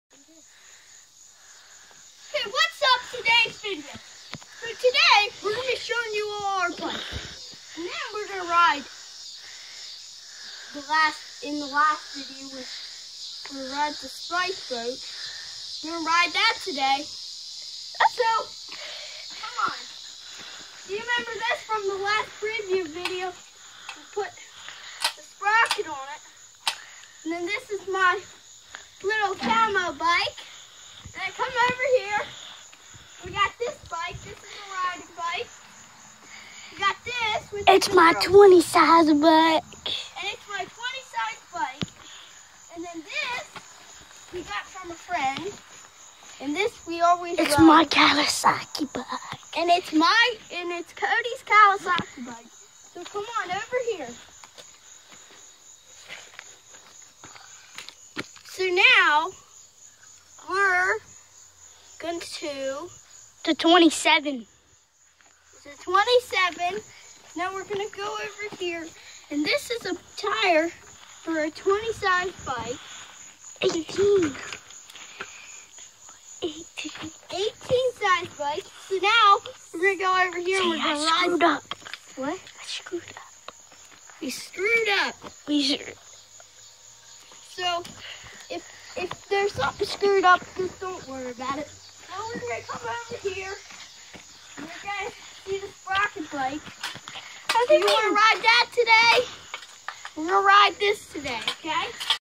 My YouTube Stars Video 2
Love my Youtube Stars. They have watched too many youtube videos and this is what they do when they steal my phone!